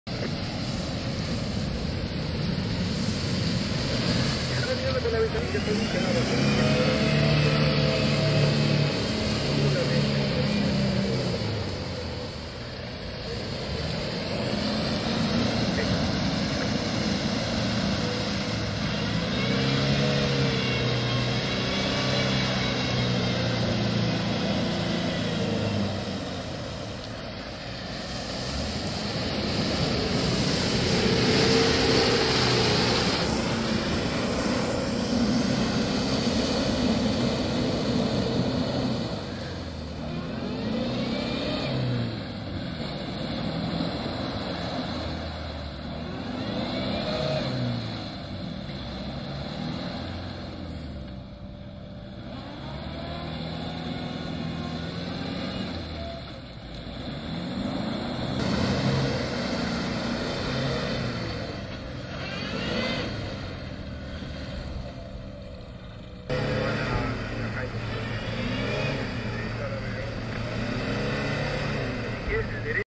SW4 intenta cruce del lodazal, gran nevada y deshielo en Laguna Epulaufquen inferior, Neuquén.
La laguna Epulafquen inferior un lago de origen glacial, en el departamento Minas, al norte de la provincia del Neuquén, Patagonia argentina.​El lago se encuentra a unos 1.472 metros sobre el nivel del mar, cerca de la frontera con Chile. Está rodeado de bosques de notofagáceas